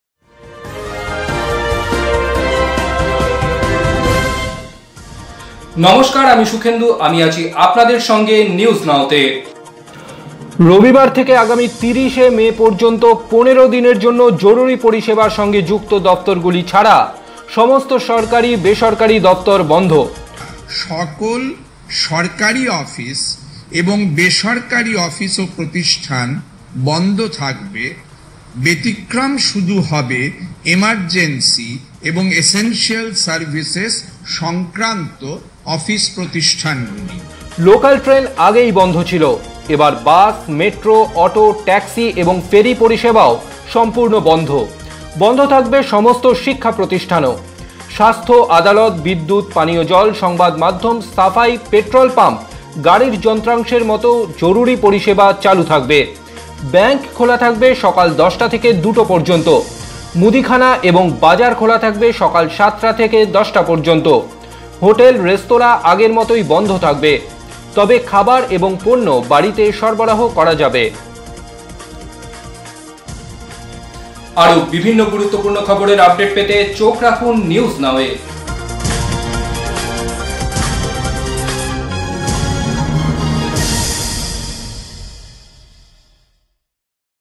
কাল থেকে রাজ্যজুড়ে লকডাউন। দেখে নিন আর কি কি বিধি নিষেধ আছে
State-wide lockdown from tomorrow. Here are the other guidelines

WATCH